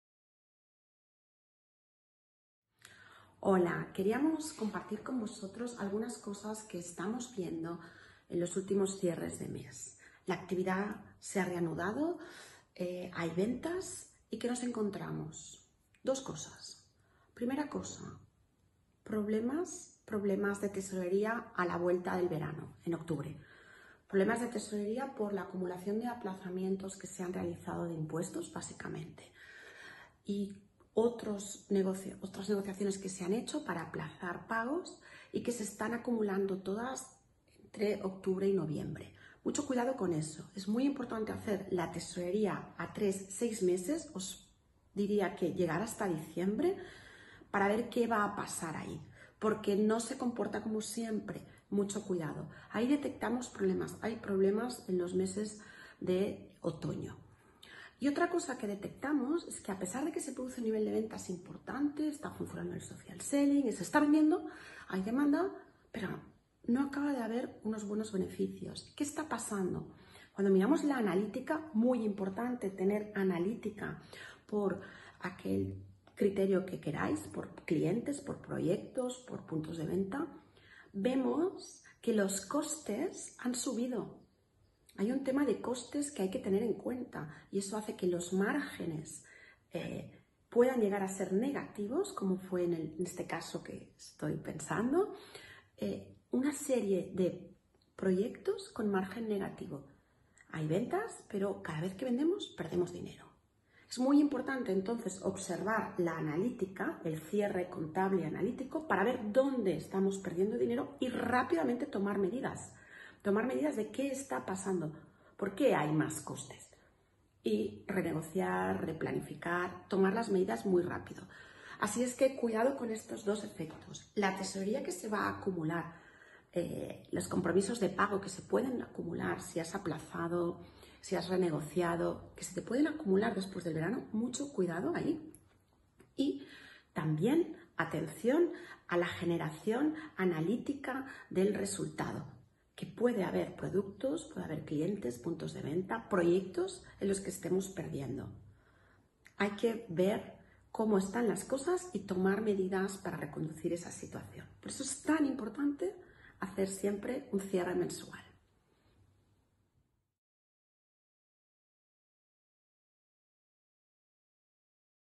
Los beneficios del cierre contable mensual
El cierre contable mensual es una práctica sencilla y al alcance de todas las empresas. La verdad es que cuantas más empresas conocemos más confirmamos la importancia de “cerrar” la contabilidad todos los meses.

Enfrentarnos con la realidad objetiva cada mes nos mantiene en forma. Nos ayuda a conocer bien la evolución del negocio y a conservar el orden, toda la organización sabe que las cosas se controlan sistemáticamente, y que sean buenas o malas, no pasan inadvertidas.

El cierre contable mensual durante la etapa post-covid es más importante que nunca. En este vídeo, Argelia Garcia, Directora Financiera y CEO de ToGrowfy, reflexiona sobre dos problemas que ha detectado analizando el cierre contable mensual de un gran número de empresas:

1. Problemas de tesorería por la acumulación de los aplazamiento de impuestos. Para tener controlado este punto es fundamental planificar la tesorería a 3/6 meses.

2. Al analizar el cierre contable analítico se ha detectado que, a pesar de las ventas, los costes han aumentado y eso provoca que los márgenes sean negativos. En algunos negocios cada vez que venden pierden dinero.

El cierre contable mensual permite a las empresas saber cómo van y, en el caso de que estén perdiendo dinero...que puedan tomar decisiones para evitarlo de forma ágil.

El cierre contable mensual es una de las buenas prácticas de la Gestión Económica Inteligente. En el blog de ToGrowfy encontrarás más información al respecto

Y si necesitas que te ayudemos, ¡contáctanos! :)